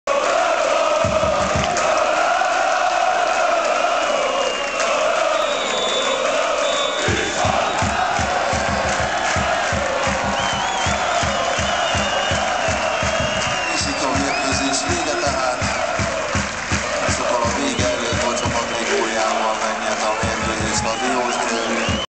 Mezőkövesd - DVTK 13/14 - A diósgyőri szurkolók
Hazai pályát teremtettek a borsodi rangadón a DVTK szurkolói Mezőkövesden.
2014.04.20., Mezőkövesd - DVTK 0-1